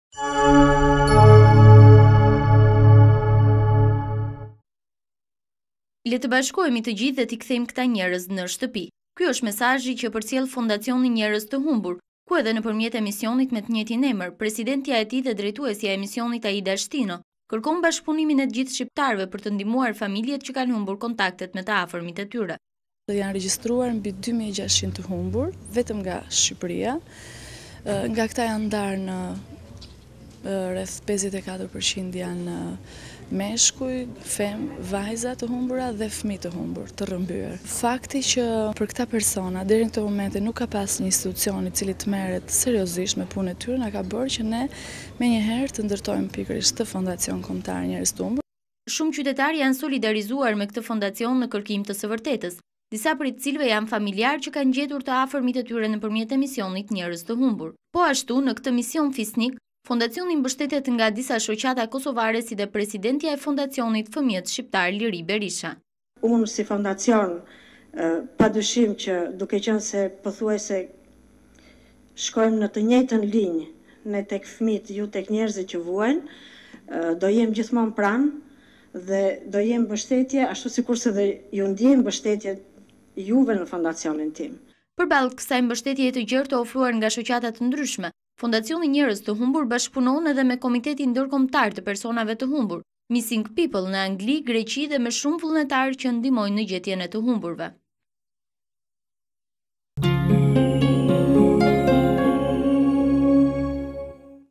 Fondacioni Njerëz të Humbur, Shtino: 2600 persona të humbur në Shqipëri-(24 Dhjetor 2008)
“Le të bashkohemi të gjithë dhe ti kthejmë këta njerëz në shtëpi”. Ky është mesazhi që përcjell fondacioni “Njerëz të humbur”, ku edhe nëpërmjet emisionit me të njëjtin emër, presidentja e këtij  fondacioni  dhe drejtuesja e emisionit, Aida Shtino, kërkon bashkëpunimin e të gjithë shqiptarëve për të ndihmuar familjet që kanë humbur kontaktet me të afërmit e tyre.

Aida Shtino: “Janë regjistruar mbi  2600 të humbur vetëm nga Shqipëria, nga këta janë ndarë në rreh 54% janë meshkuj, vajza të humbura dhe fëmijë të humbur, të rrëmbyer. Fakti që për orta persona deri në këta momente nuk ka pasur një institucion i cili të merret seriozisht me punën e tyre na ka bërë që ne menjëherë të ndërtojmë pikërisht këtë fondacion kombëtar Njerëz të Humbur.”

Shumë qytetarë janë solidarizuar, me këtë fondacion në kërkim të së vërtetesës, disa prej të cilëve janë familjar që kanë gjetur të afërmit e tyre nëpërmjet emisionit Njerëz të Humbur. Po ashtu në këtë  mision  fisnik, fondacionit mbështetet disa shoqata kosovare si dhe nga presidentja e fondacionit Fëmijët Shqiptar, Liri Berisha.

Liri Berisha: “Unë si fondacion duke qenë se pothuajse shkojmë në të njëjtën linjë, ne tek fëmijët, ju tek njerëzit që vuajnë. Do jem gjithmonë pranë dhe do jem mbështetja ashtu sikurse dhe ju ndjej mbështetje ju dhe në fondacionin tim.”

Përballë kësaj mbështetje të gjerë të ofruar nga shoqata të ndryshme fondacioni Njerëz të Humbur, bashkëpunon edhe me komitetin ndërkombëtar të personave të humbur “Mising People” në Angli, Greqi dhe me shumë vullnetarë që ndihmojnë në gjetjen e të humburve./tvklan.al

Gazetare: Brunilda Cami